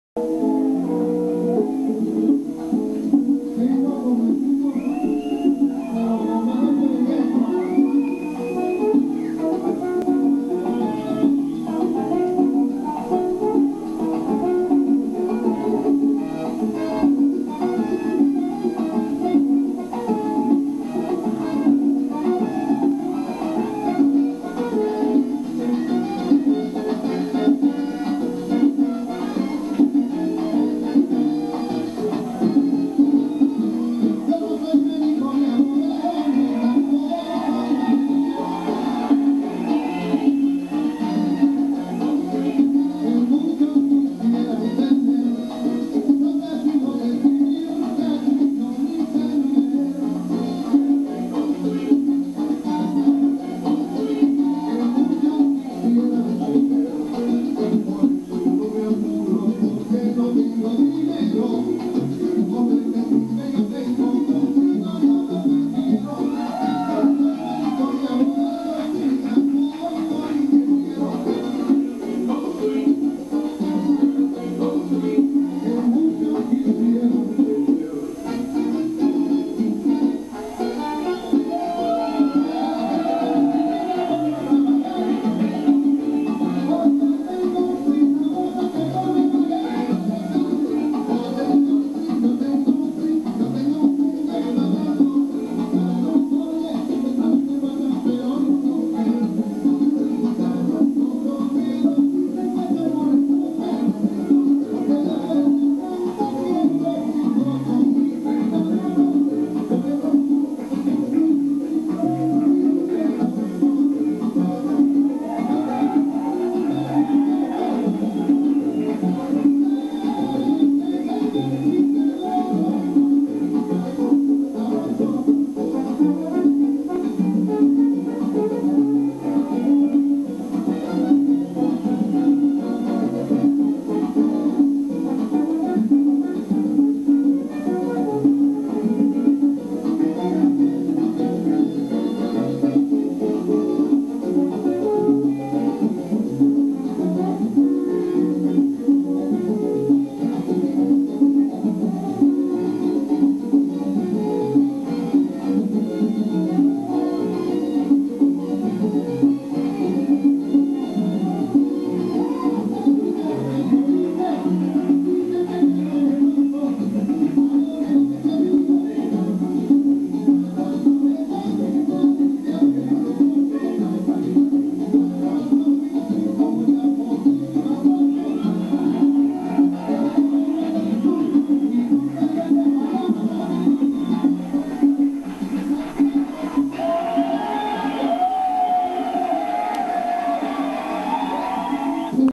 gil et patricia
gil and patricia dancing salsa on 2, salsatech, montreal, canada, december 1996